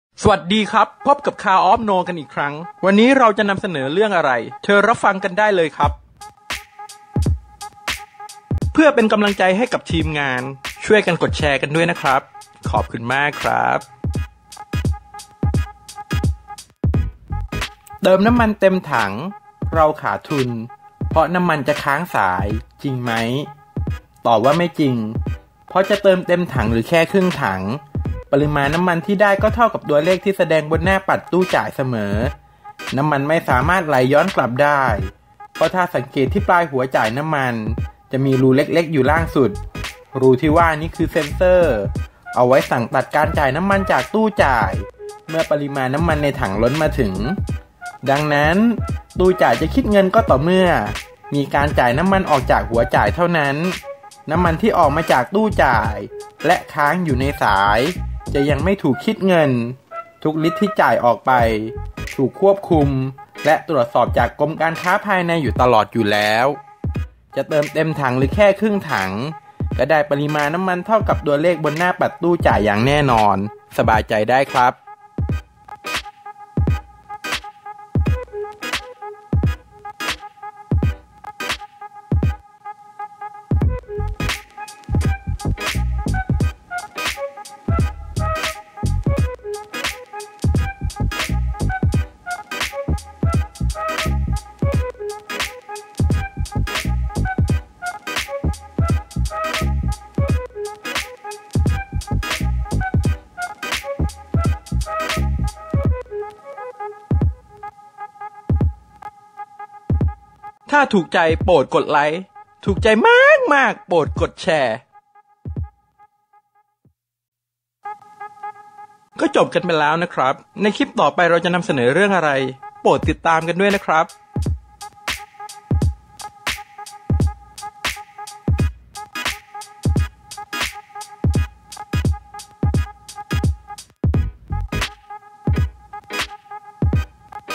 จริงหรือไม่ที่เติมน้ำมันเต็มถัง ขาดทุนเพราะน้ำมันค้างสาย : Car of Know
อุปกรณ์มอเตอร์ไซค์(Motorcycle Accessories) ชิ้นส่วนอะไหล่รถยนต์ หรือ มอเตอร์ไซค์ ที่เกิดความ